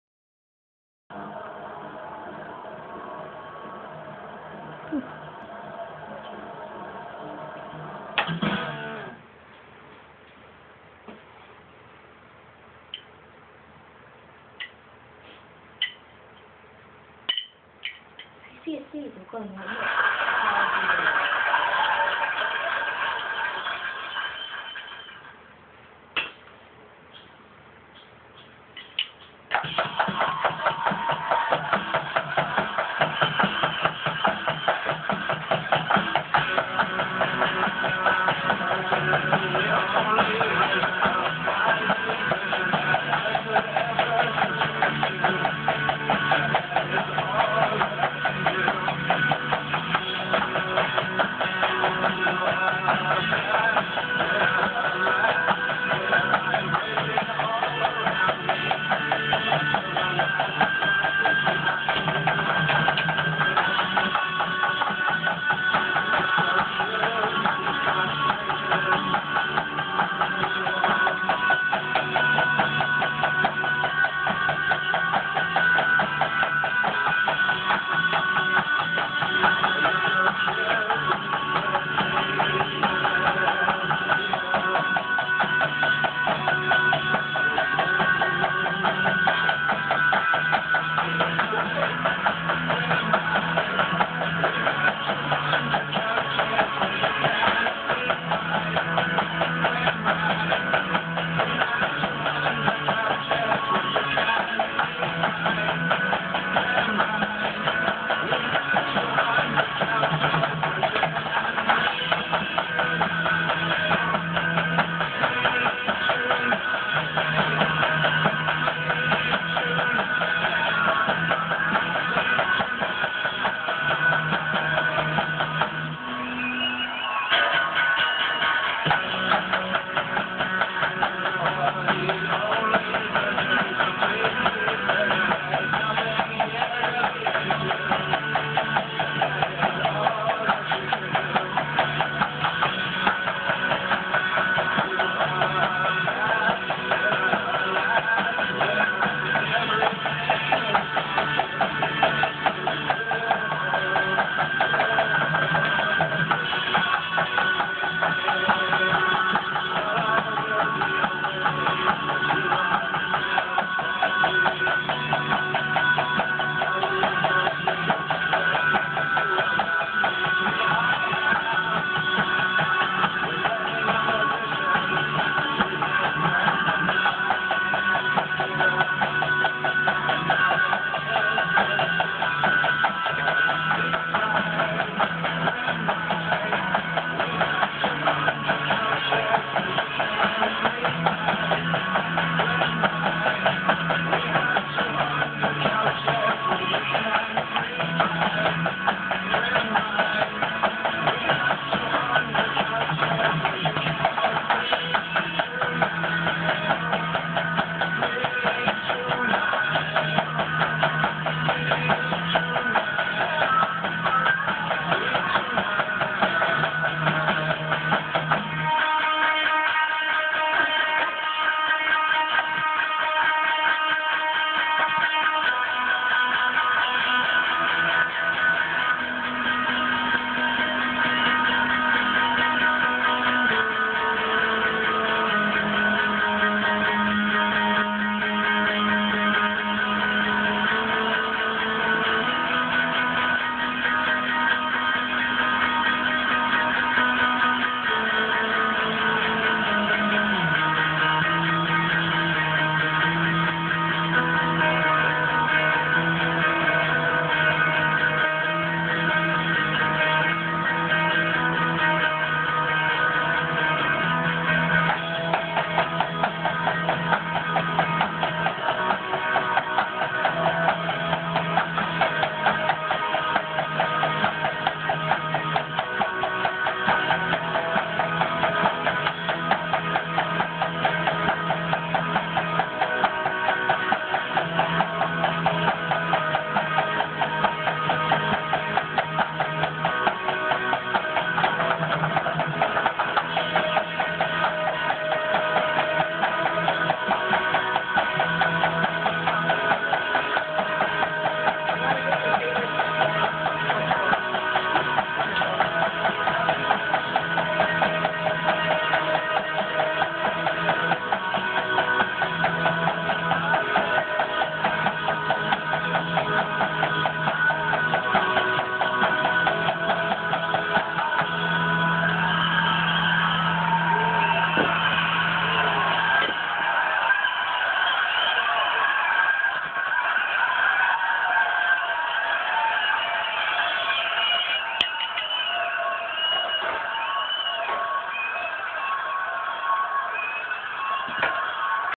Me not sucking so much on drums
An easier song lol I still suck though. I'm not to Goo with the drums.